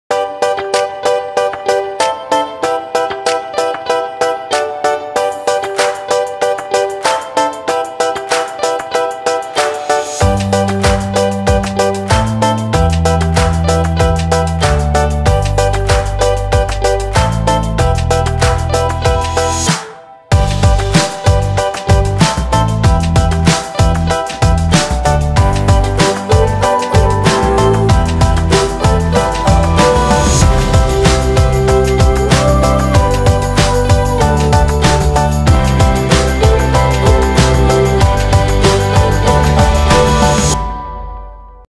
德國【Invisibobble】魔髮小丸子造型圈使用教學
Invisibobble 魔髮小丸子造型圈 終於引進台灣了!
跟著使用教學影片一起打造最完美的丸子頭